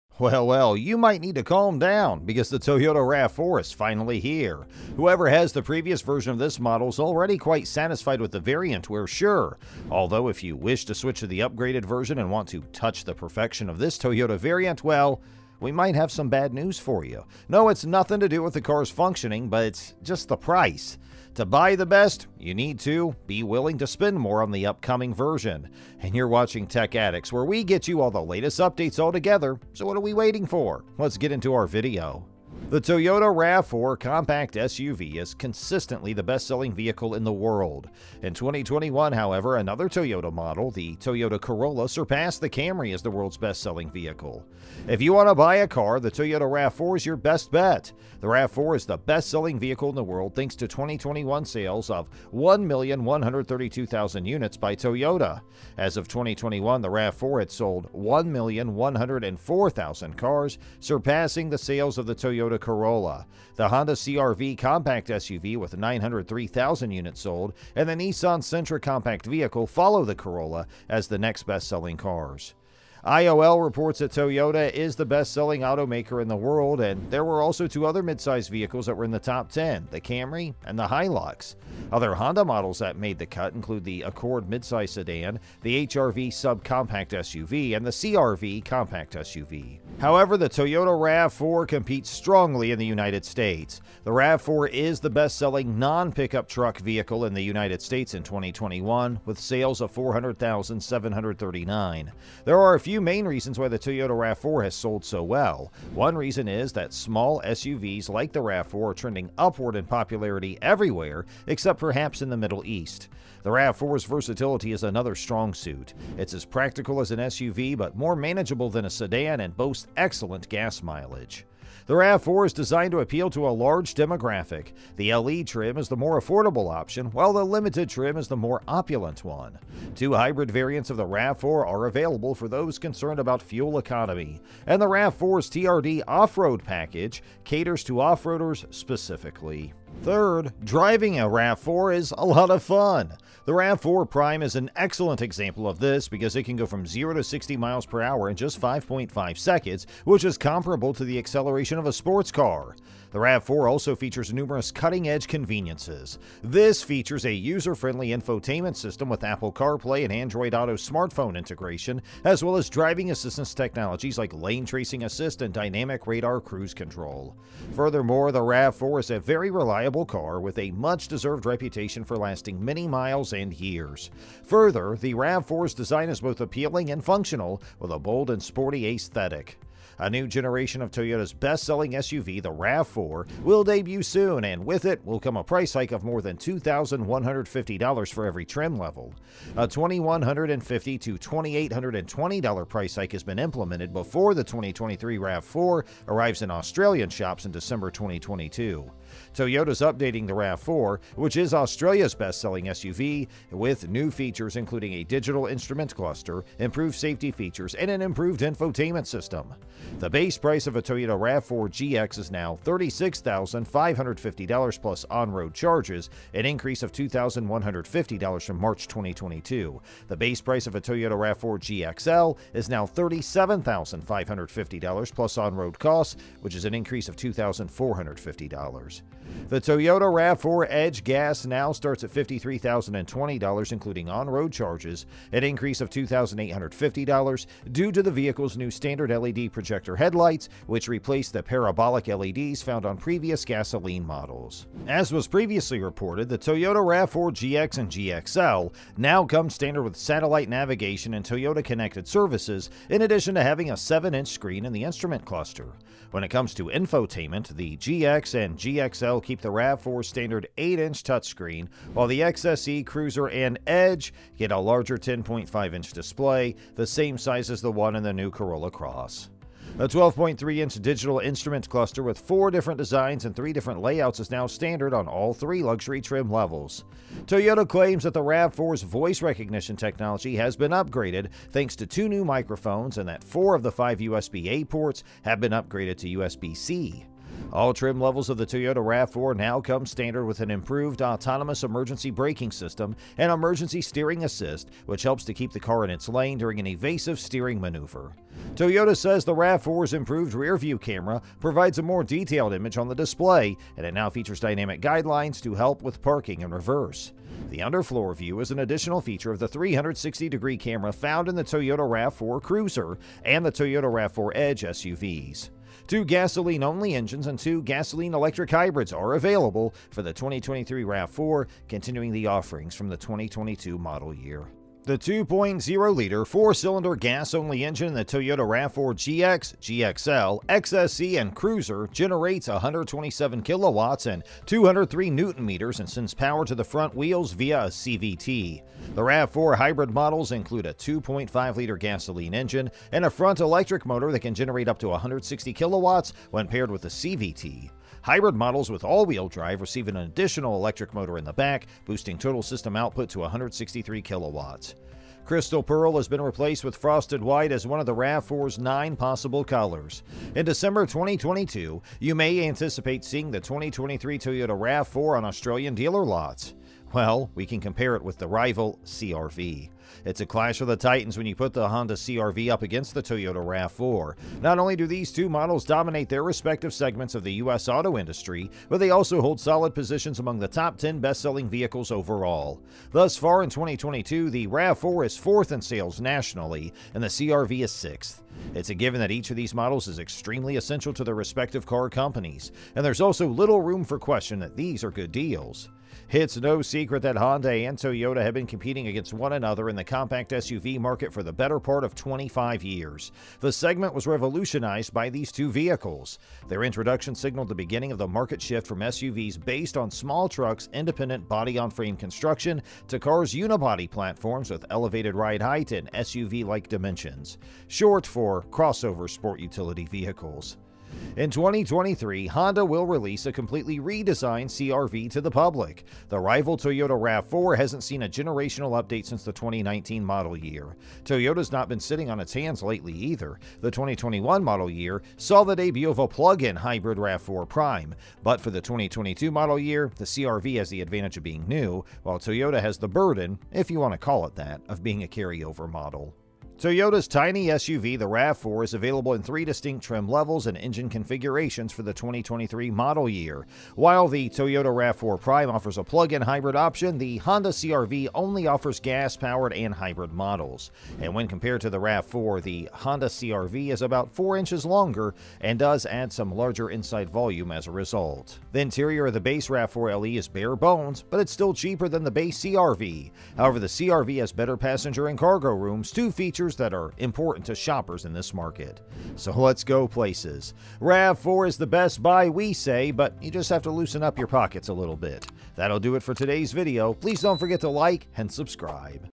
This is BAD NEWS For The 2023 Toyota RAV4 Owners!
Well well, you need to calm down first as the RAV4 is finally here, whoever has the previous version of this model is already quite satisfied with this variant, although if you wish to switch to the upgraded version and want to “Touch the Perfection” of this Toyota variant, well, we have bad news for you, no no nothing related to the car’s functioning, but with the price! Well, to buy the best you need to and be willing to spend more on the upcoming one! You're watching TECH ADDICTS, where we get you all the latest updates altogether, so what are we waiting for, let's get into the video!

The RAV4 compact SUV is consistently the best-selling vehicle in the world. In 2021, however, another Toyota model, the Toyota Corolla surpassed the Camry as the world's best-selling vehicle.

If you want to buy a car, the Toyota RAV4 is your best bet. The RAV4 is the best-selling vehicle in the world thanks to 2021 sales of 1,132,000 units by Toyota. As of 2021, the Toyota RAV4 had sold 1,104,000 cars, surpassing the sales of the Toyota Corolla by a wide margin. The Honda CR-V compact SUV (903,000 units sold) and the Nissan Sentra compact vehicle follow the Corolla as the next best-selling cars (693,000 units sold).

IOL reports that Toyota is the best-selling automaker in the world. There were also two other midsize vehicles in the top 10: the Camry and the Hilux. Other Honda models that cut include the Accord midsize sedan, the HR-V subcompact SUV, and the CR-V compact SUV.

However, the Toyota RAV4 competes strongly in the United States. The RAV4 is the best-selling non-pickup truck vehicle in the United States in 2021, with sales of 407,739.

There are a few main reasons why the Toyota RAV4 has sold so well. One reason is that small sport utility vehicles (SUVs) like the Toyota RAV4 are trending upward in popularity everywhere except perhaps in the Middle East. The RAV4's versatility is another strong suit. It's as practical as an SUV but more manageable than a sedan and boasts excellent gas mileage.

The RAV4 is designed to appeal to a large demographic. The LE trim is the more affordable option, while the Limited trim is the more opulent one. Two hybrid variants of the RAV4  are available for those concerned about fuel economy. And the RAV4's TRD Off-Road package caters to off-roaders specifically.

Third, driving a RAV4 is a lot of fun. The RAV4 Prime is an excellent example of this because it can go from 0 to 60 mph in just 5.5 seconds, which is comparable to the acceleration of a sports car.

The RAV4 also features numerous cutting-edge conveniences. This features a user-friendly infotainment system with Apple CarPlay and Android Auto smartphone integration, as well as driving assistance technologies like lane-tracing assist and dynamic radar cruise control.

Furthermore, the RAV4 is a very reliable car, with a much-deserved reputation for lasting many miles and years. Further, the RAV4's design is both appealing and functional, with a bold and sporty aesthetic.

A new generation of Toyota's best-selling SUV, the RAV4, will debut soon, and with it will come a price hike of more than $2150 for every trim level.

A $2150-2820 price hike has been implemented before the 2023 Toyota RAV4 arrives in Australian shops in December 2022.

Toyota is updating the RAV4, Australia's best-selling SUV, in 2023 with new features including a digital instrument cluster, improved safety features, and an improved infotainment system.

The base price of a Toyota RAV4 GX is now $36,550 plus on-road charges, an increase of $2150 from March 2022; the base price of a Toyota RAV4 GXL is now $37,550 plus on-road costs, an increase of $2450.

The Toyota RAV4 Edge petrol now starts at $53,020 including on-road charges, an increase of $2850 due to the vehicle's new standard LED projector headlights, which replace the parabolic LEDs found on previous gasoline models.